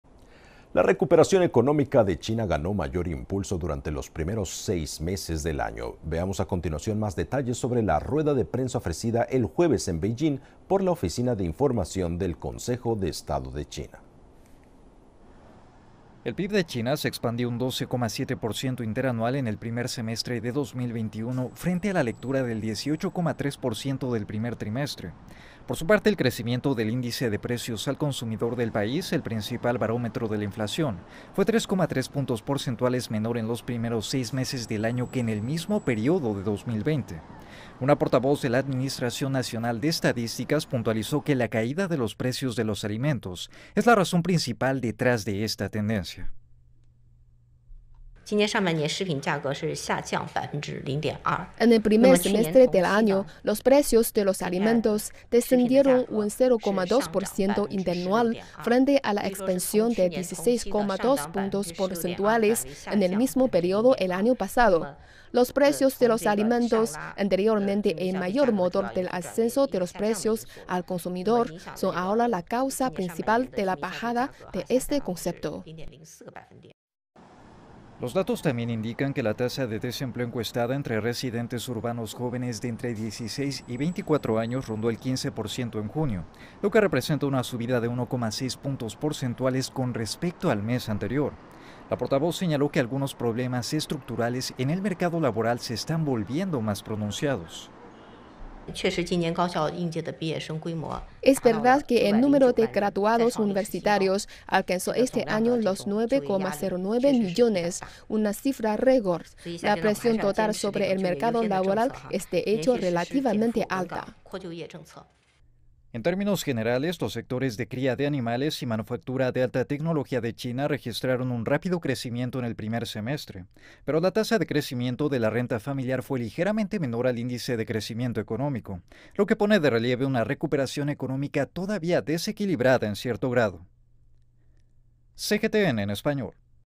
El crecimiento del IPC de China se ralentiza en medio de una recuperación desequilibrada
La recuperación económica de China ganó mayor impulso durante los primeros seis meses del año.